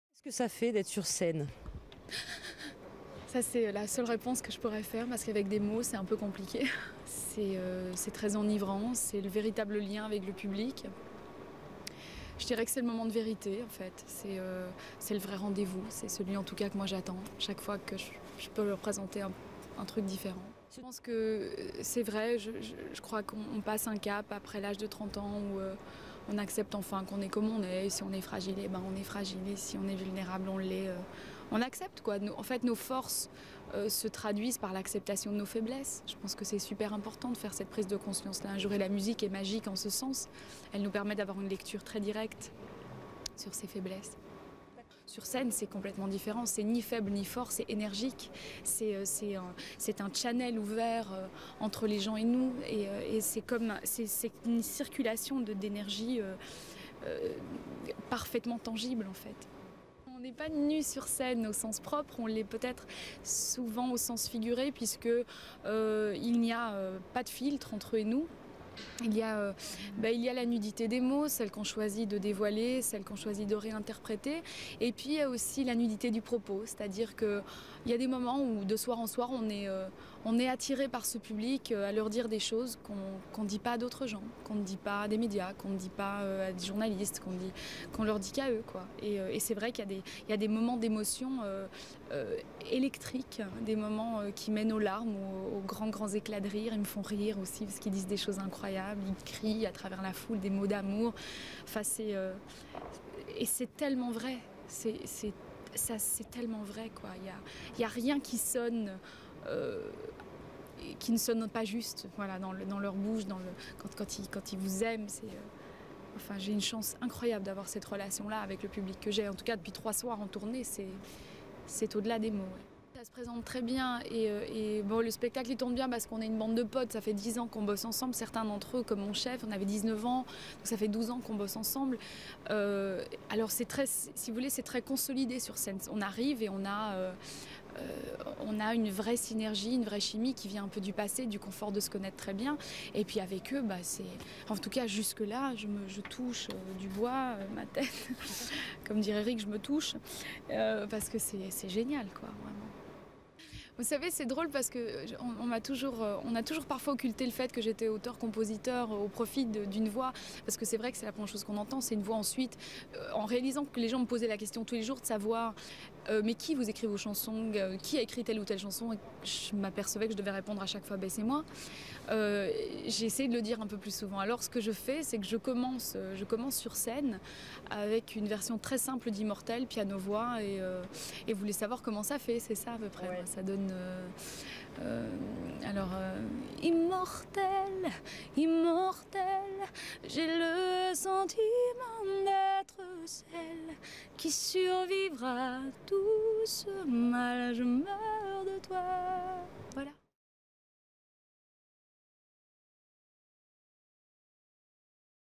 Extrait archives M6 Video Bank // Lara Fabian - 2002
Extrait d’une interview de Lara Fabian de 2002. Elle revenait sur le sentiment d’être sur scène, le cap des 30 ans, l’acceptation de ses faiblesses…